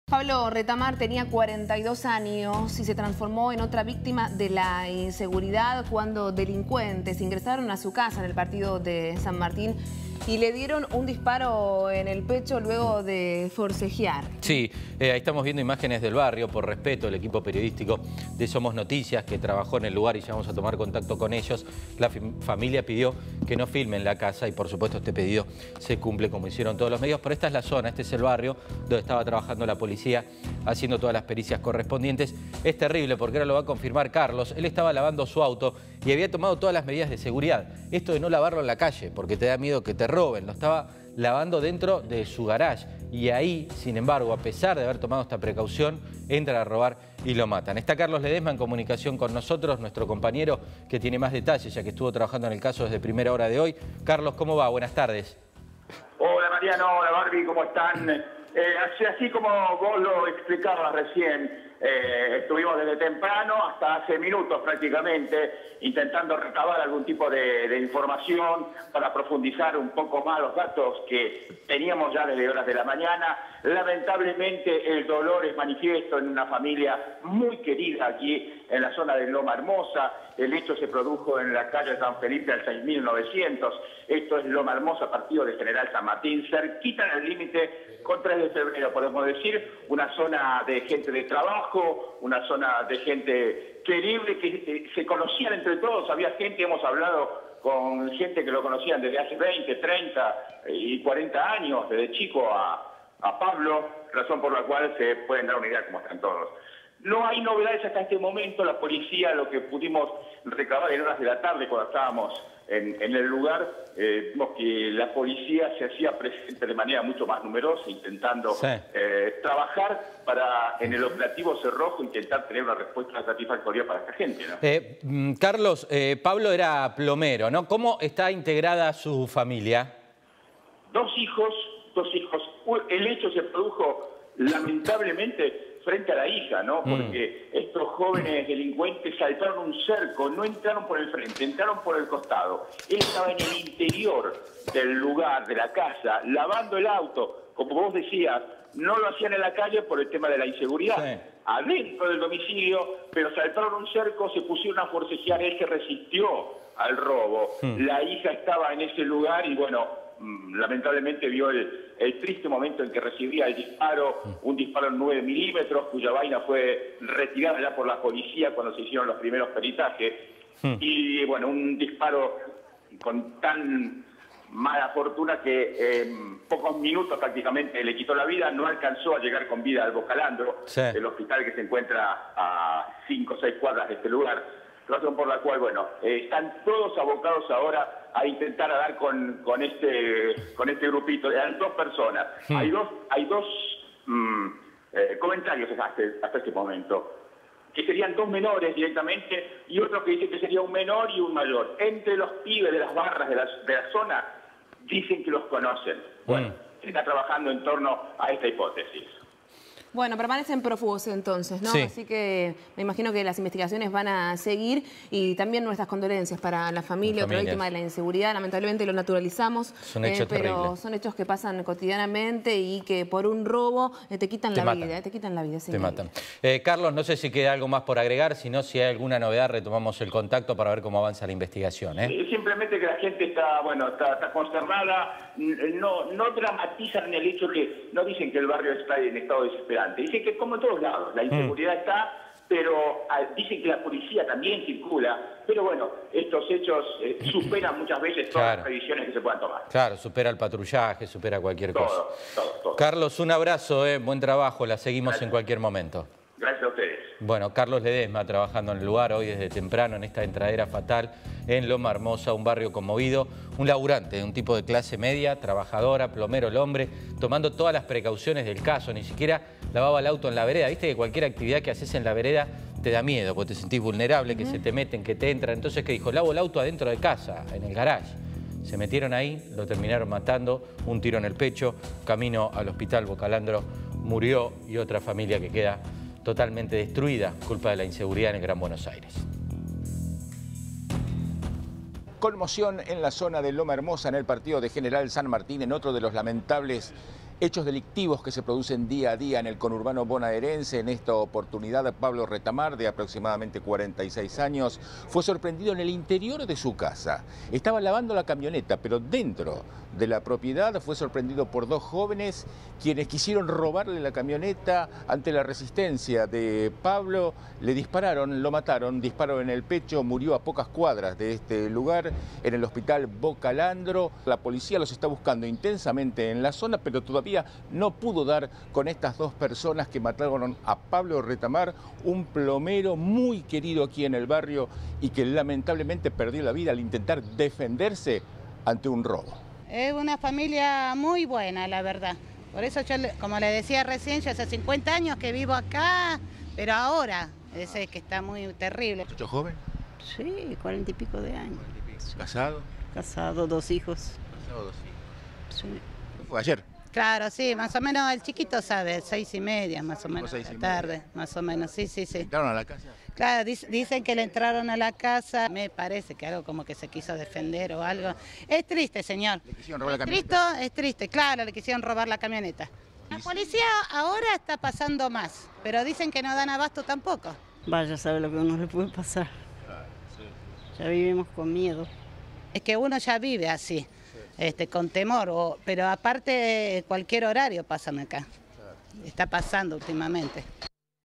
ENTRADERA FATAL EN SANMARTÍN
Pablo Retamar, de 42 años, murió de un tiro en el pecho delante de su hija luego de forcejear con dos delincuentes que entraron a su casa con fines de robo en San Martín. El hecho ocurrió ayer en San Felipe al 6900, Loma Hermosa, donde dos asaltantes, menores de edad, ingresaron al patio de la vivienda de Pablo Retamar, un plomero de 42 años. Al descubrir a los delincuentes, Retamar forcejeó con ellos y recibió un tiro. La víctima murió camino al hospital Bocallandro. Los ladrones, de 15 y 17 años, permanecen prófugos e interviene en la causa personal de la Comisaría 5ta de San Martín, que secuestró una vaina de 9 mm. en el lugar.